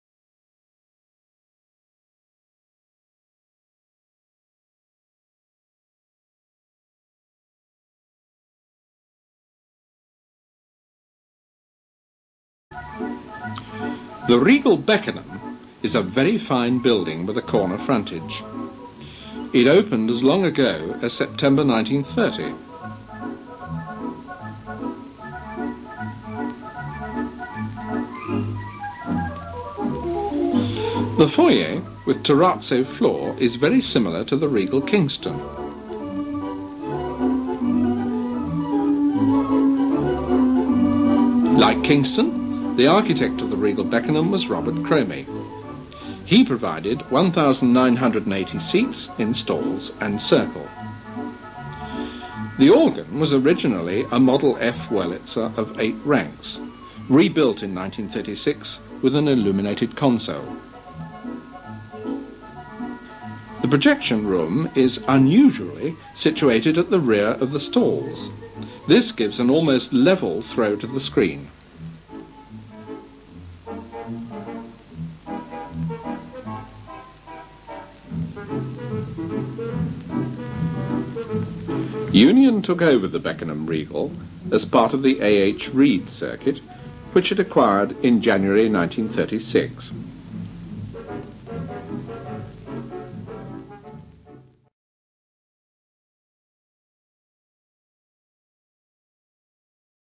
MOVIE PALACES #51 - THE REGAL BECKENHAM KENT - 1930
A quick look at the REGAL CINEMA, BECKENHAM, Opened in 1930.
Video uploaded by ComptonLodgeStudios, Compton Lodge, Sapcote, Leicester United Kingdom.